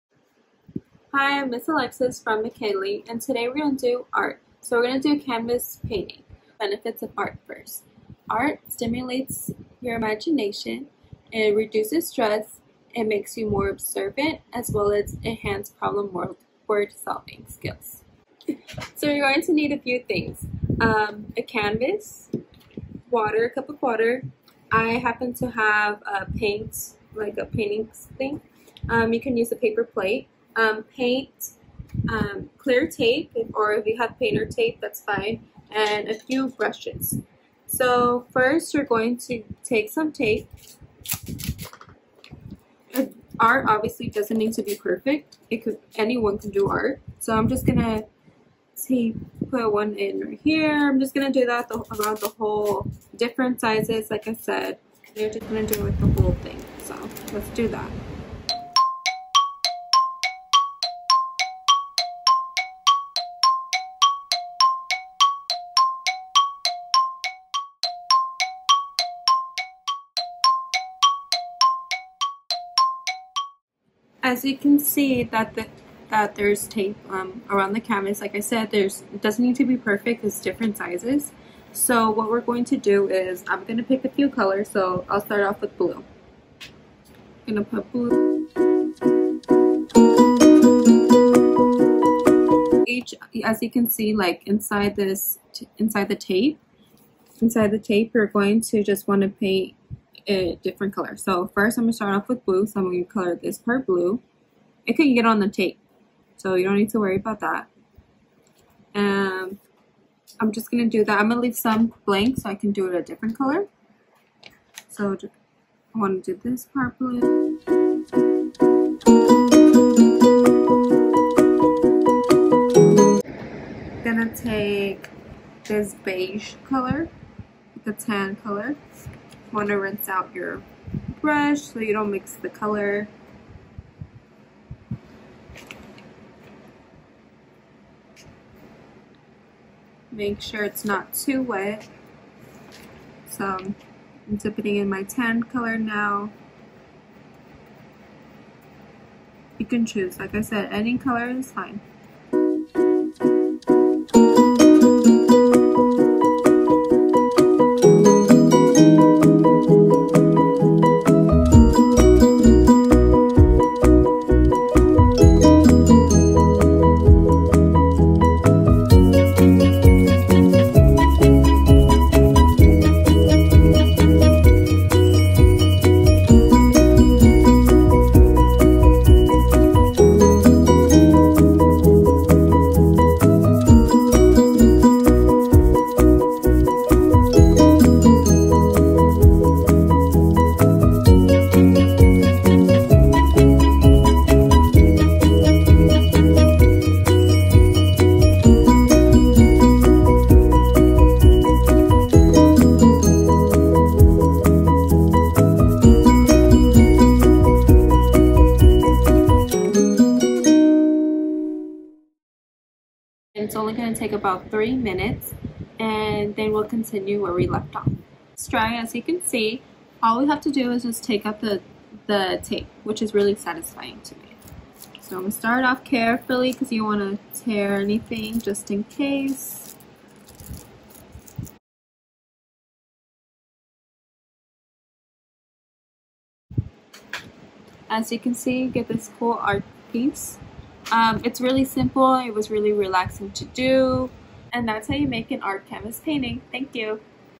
Art Canvas Painting w/Alexis, Program Specialist
Watch this video and learn to make a art canvas painting!